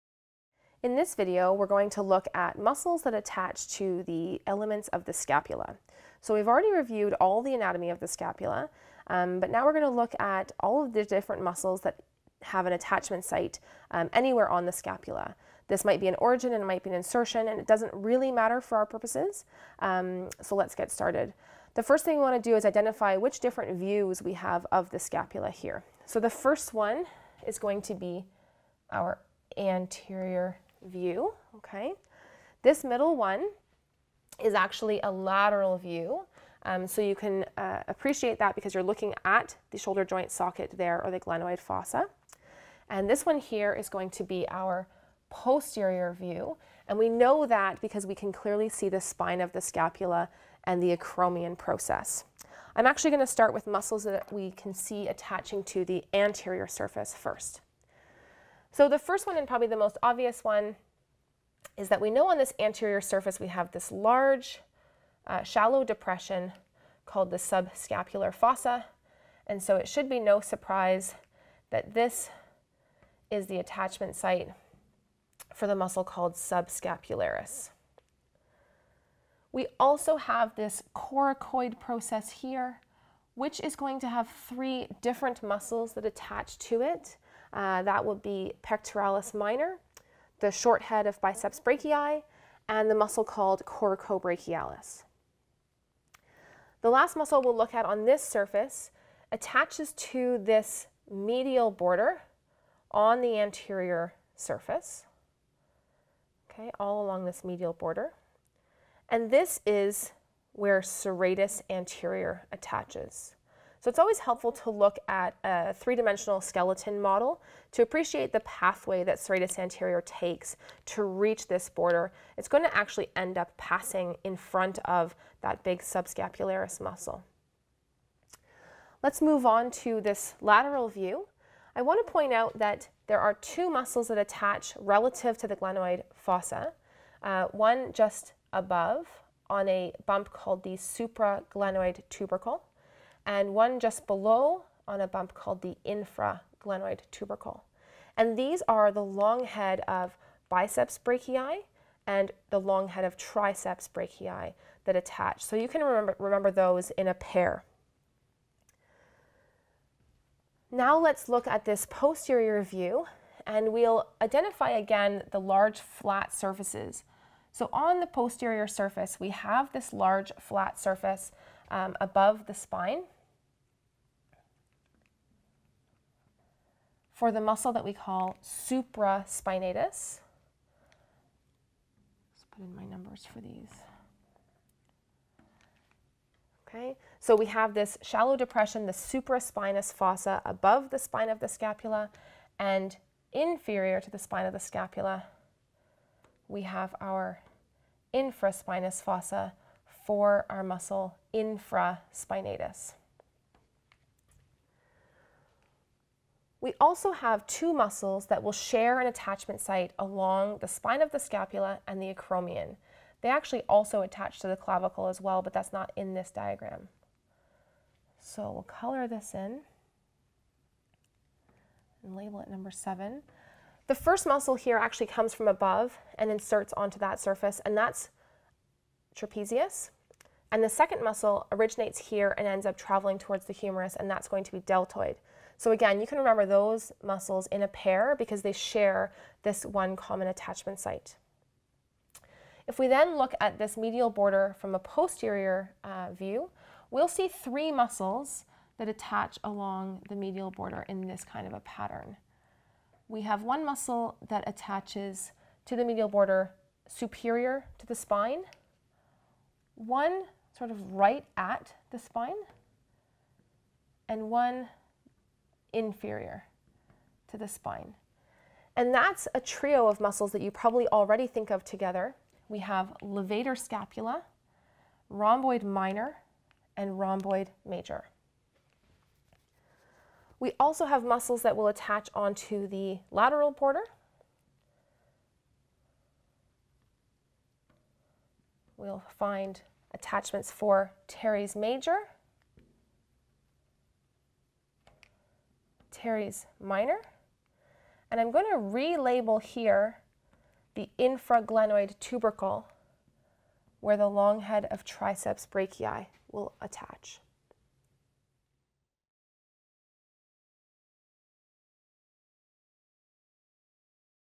Muscles Attaching to the Scapula
Legend:
1. Subscapularis
2. Short head of biceps brachii, pectoralis minor, and coracobrachialis
Serratus anterior (labeled but not numbered)
3. Long head of biceps brachii
4. Long head of triceps brachii
5. Supraspinatus
6. Infraspinatus
7. Trapezius and deltoid
8. Levator scapula
9. Rhomboid minor
10. Rhomboid major
11. Teres major
12. Teres minor